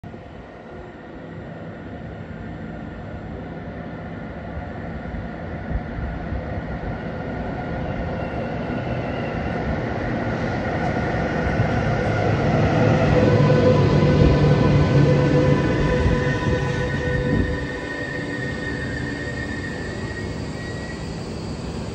Einfahrt 101 005 (DEVK Folierung), Darmstadt Hbf
101 005, welche Grün foliert ist mit Werbung der DEVK Versicherung fährt in Darmstadt Hbf ein.

A German Class 101, which is green film fittet with advertising from DEVK Insurance arrives at Darmstadt Central Station.